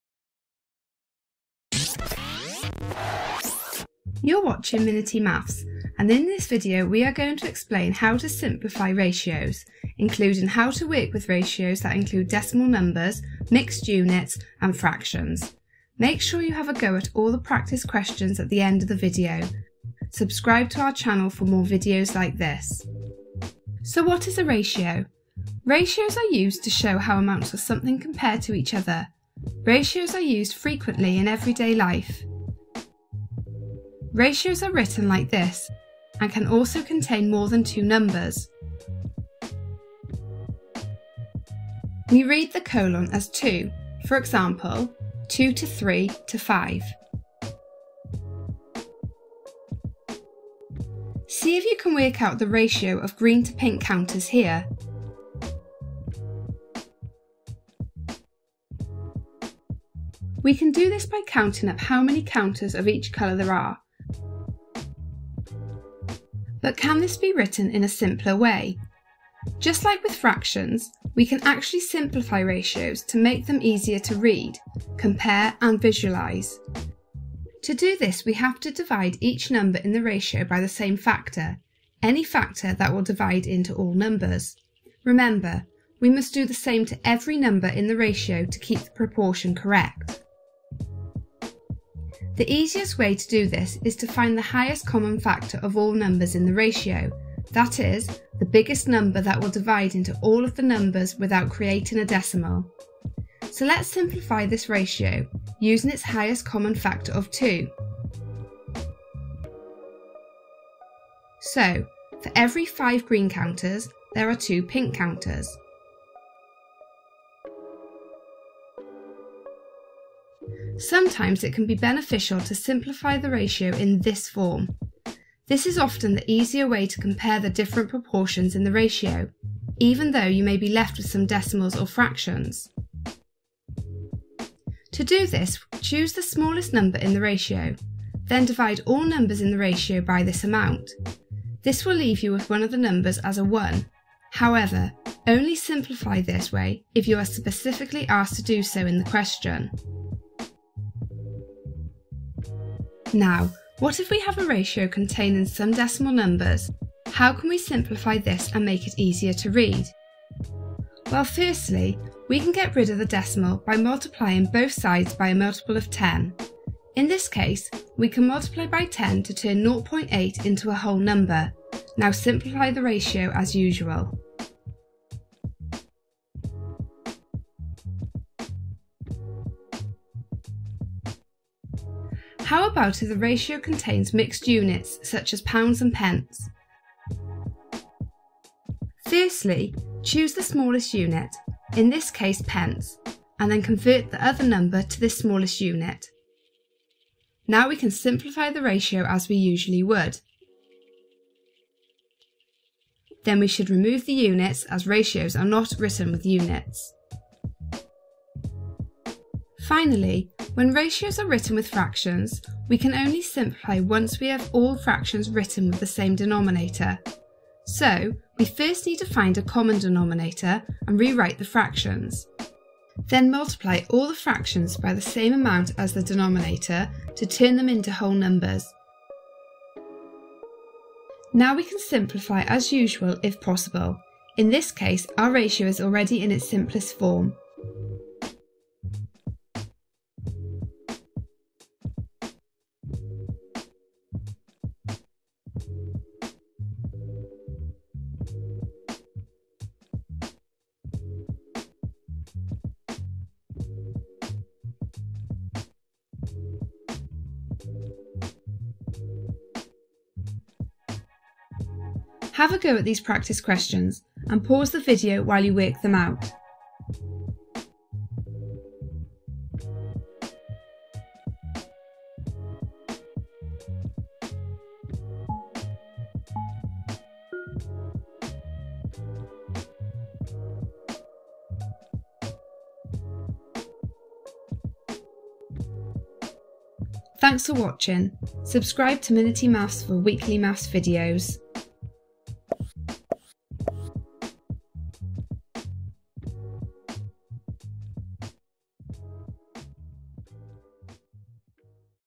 How To Simplify Ratios
This video explains how to simplify ratios, including how to simplify ratios that include decimal numbers, mixed units and fractions. We also explain how to get a ratio in the form 1:n or n:1. Make sure you have a go at all the practice questions at the end of the video.

Skip to:
00:00 Introduction
00:24 What is a ratio
00:50 How to simplify ratios
01:59 How to get ratios in the form 1:n or n:1
02:30 Simplifying ratios containing decimals
03:03 Simplifying ratios containing mixed units
03:30 Simplifying ratios containing fractions
03:59 Summary
04:24 Practice questions and answers